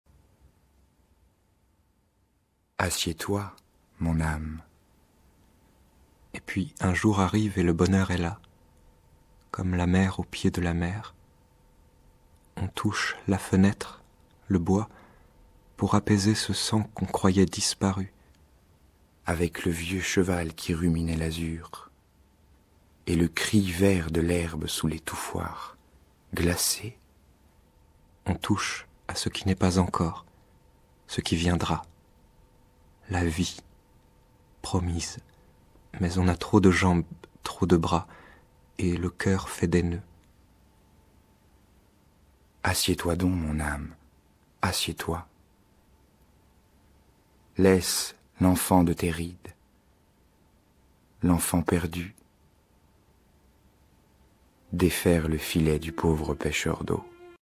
Le pêcheur d'eau, Guy Goffette
lu par Tristan Rotu et Thomas Pasquelin

Le pêcheur d'eau

Assieds-toi, mon âme

Et puis un jour arrive et le bonheur est là
comme la mer au pied de la mer, on touche
la fenêtre, le bois, pour apaiser ce sang
qu'on croyait disparu

avec le vieux cheval qui ruminait l'azur,
et le cri vert de l'herbe sous l'étouffoir
glacé ; on touche à ce qui n'est pas encore,
ce qui viendra : la vie

promise, mais on a trop de jambes, trop
de bras et le cœur fait des nœuds
- assieds-toi donc mon âme, assieds-toi, laisse
l'enfant de tes rides, l'enfant perdu

défaire le filet du pauvre pêcheur d'eau.